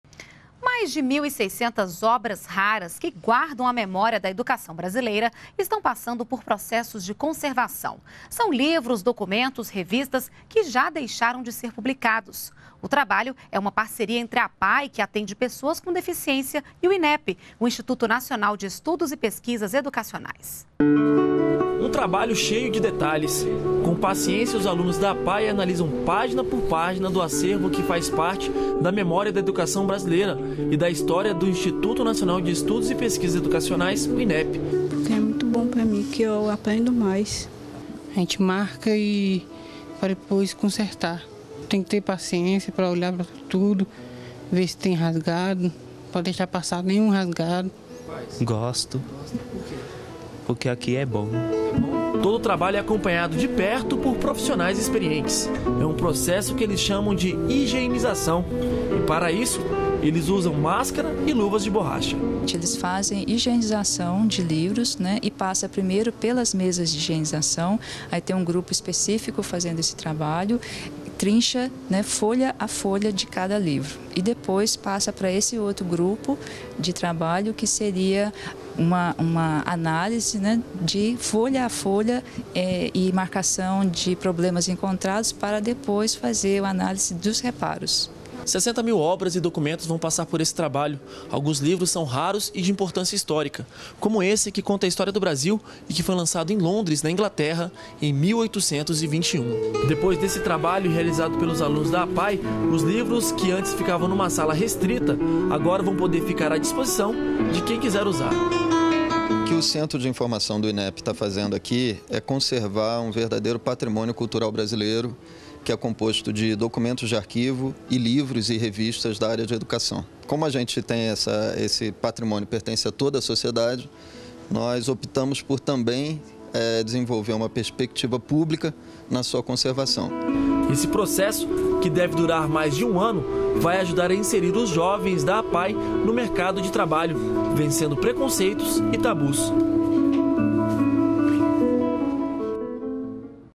Alunos da APAE trabalham na preservação de documentos raros sobre a educação brasileira
NBR NOTÍCIAS - 07.02.13: Um parceria entre a Associação de Pais e Amigos dos Excepcionais (APAE) e o Instituto Nacional de Estudos e Pesquisas Educacionais (INEP) tem o objetivo de conservar obras que fazem parte da memória da educação brasileira. A atividade realizada pelos alunos da APAE é analisar as páginas de livros, revistas e documentos de arquivo raros, uma forma também de inserir esses jovens no mercado de trabalho.